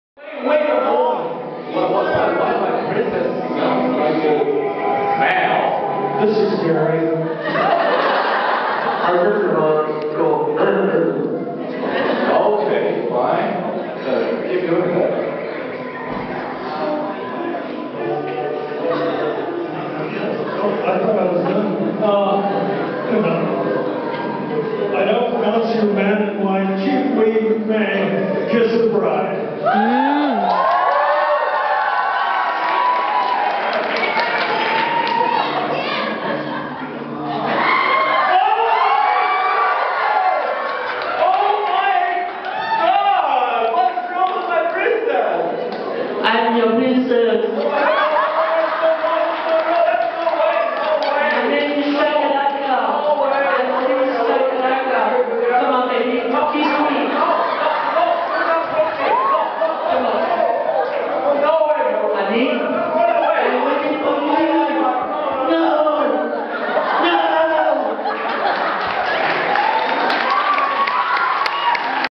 Witoto Wedding Ceremony
Wedding Ceremony at César Ritz Colleges Switzerland produced by Witoto Event Productions